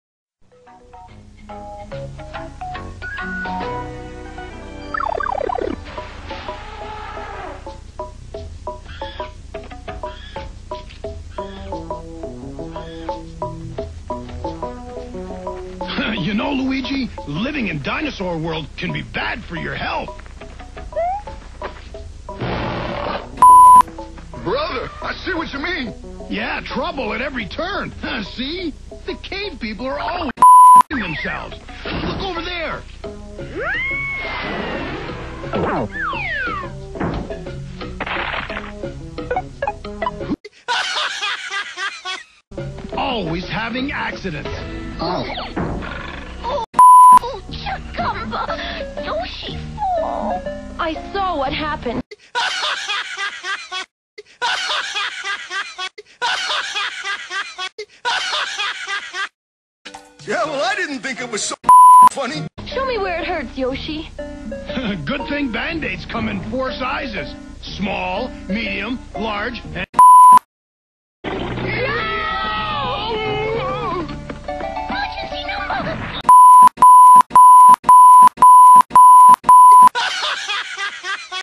YouTube Poop: Mario Hates Yoshi (TEST VERSION)
This is just a test to see if my Movie Maker is working or not! It is! :) Yeah, I know you guys are getting pissed with my YouTube Poops just being mindless clips with bleeps but I promise my next video will more than make up for it. I might later make a real version of Mario Hates Yoshi. Check back for updates!